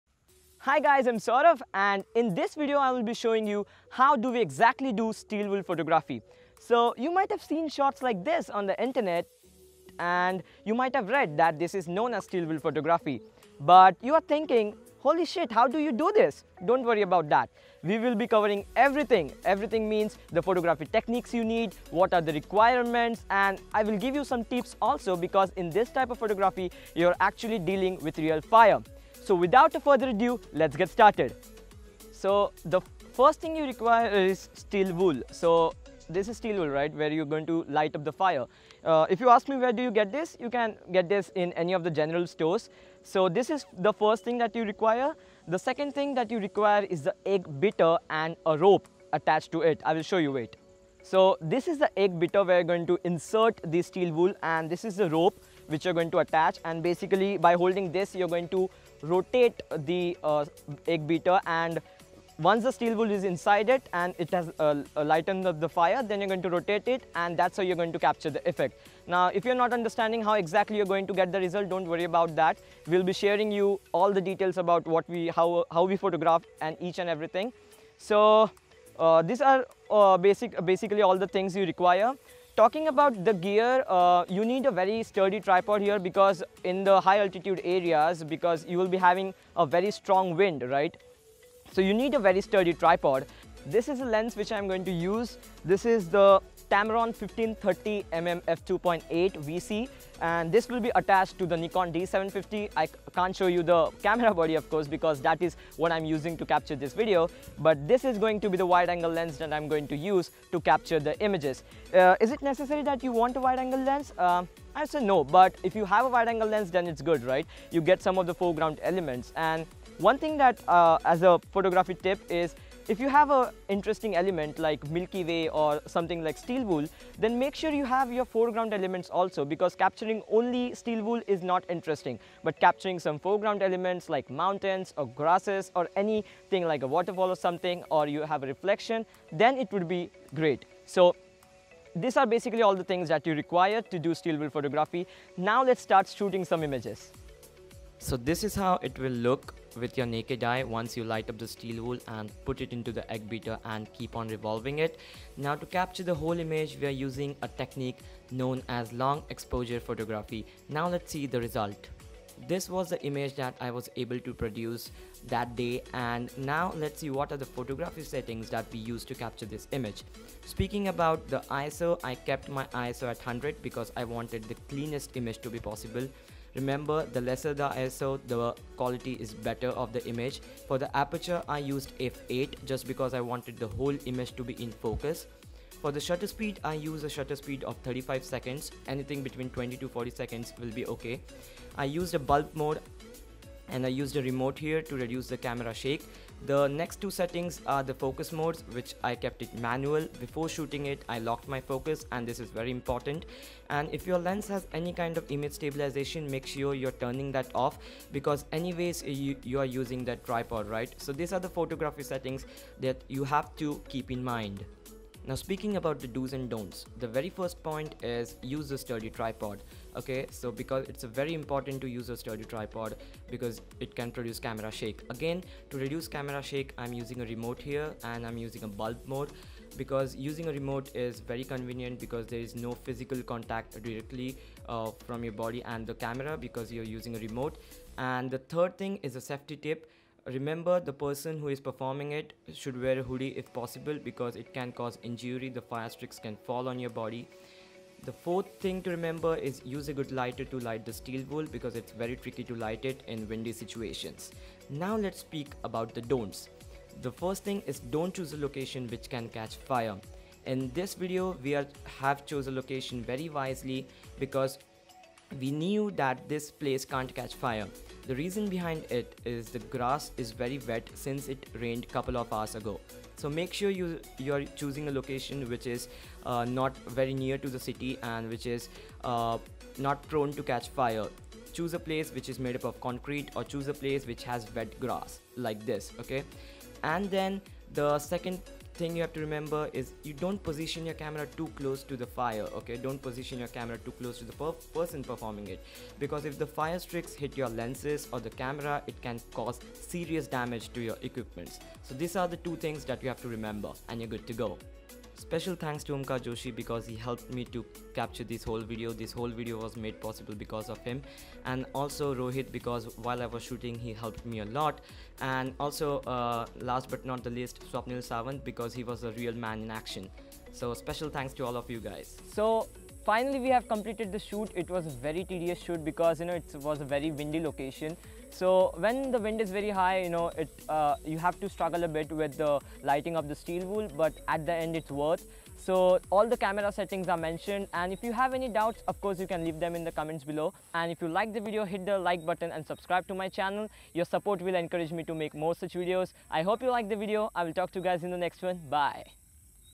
Steel Wool Photography Tutorial | HD
This photography video tutorial will explain you how to burn and spin steel wool to create super cool long exposure pictures of fire sparks also known as Steel Wool Photography.

Gear used : Nikon D750, Tamron 15-30mm f/2.8 VC, Nikon D7000, 50mm f/1.8g (for video)

Keywords (Ignore) :

steel wool,photography,tutorial,nikon,canon,tamron,light,painting,art,awesome,amazing,explaination,video,photo,fire